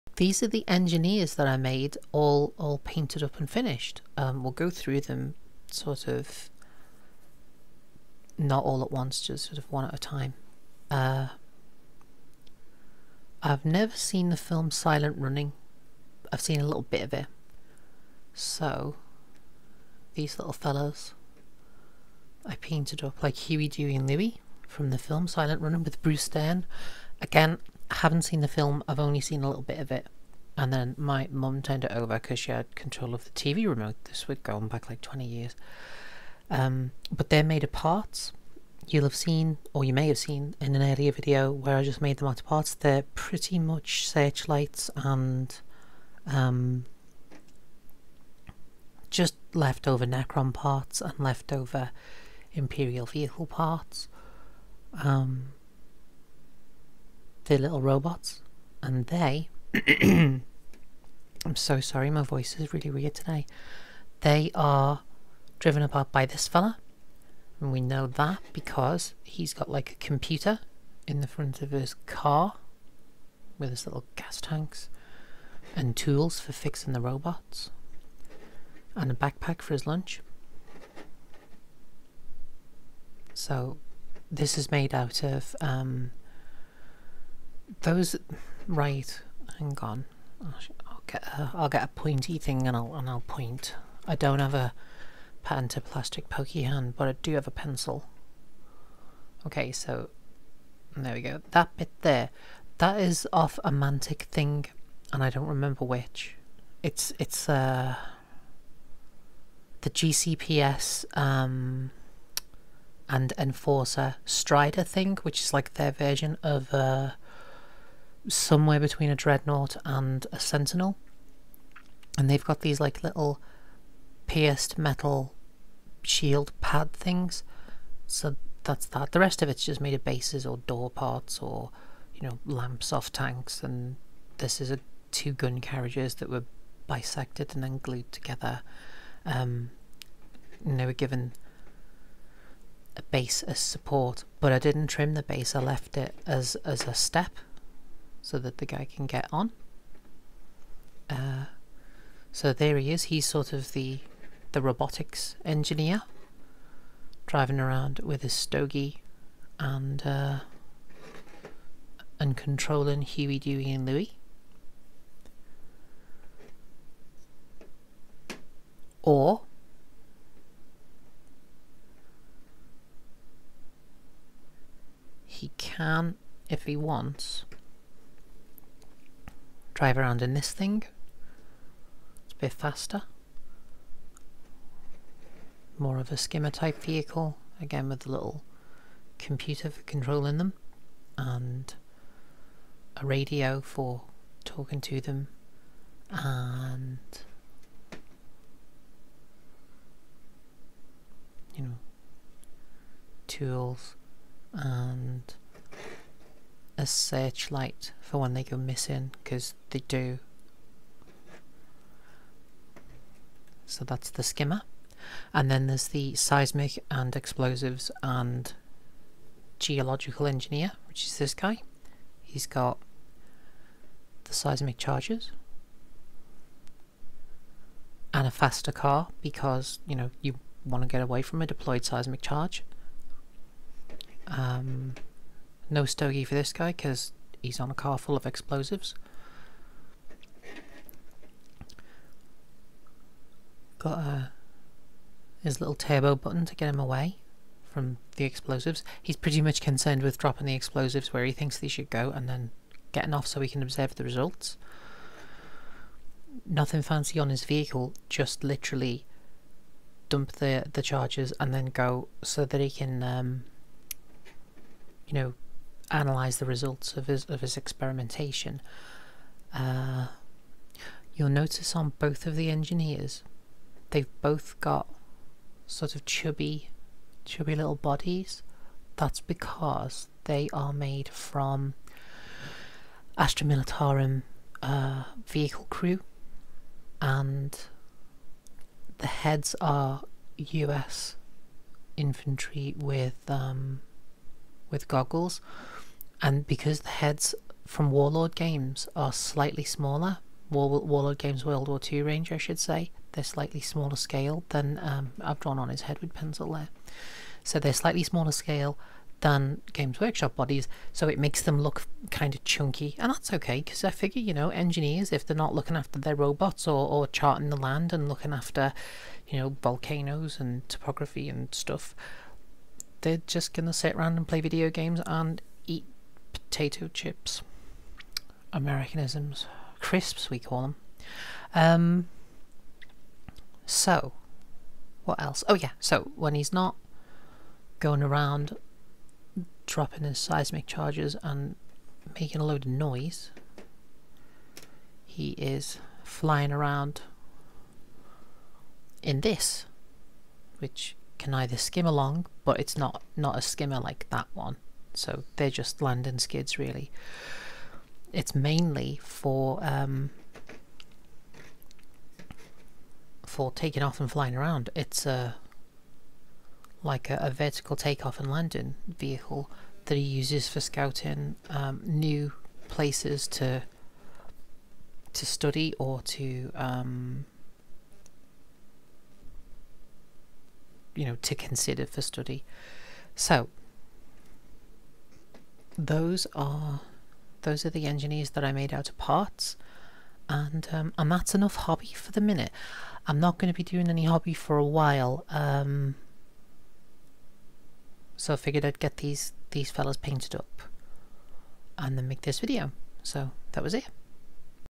Completed Kitbashed Engineers
The kitbashed engineers from an earlier video all finished!
Made from mostly GW parts with some Warlord WWII and Napoleonic stuff with a few Mantic parts thrown in.